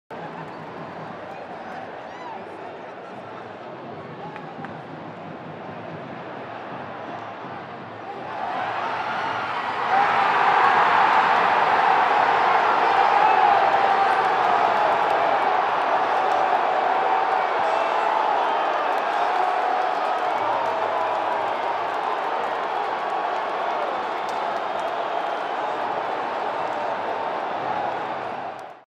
Rafael Marques aproveita sobra e marca para o Botafogo
Após chute de longe, Rafael Marques dominou a sobra livre na área e tocou na saída do goleiro Jefferson para abrir o placar para o Botafogo no clássico contra o Vasco

Cliente: UOL
Serviço: Captação de imagens e edição
Realização: NNS FILMES ®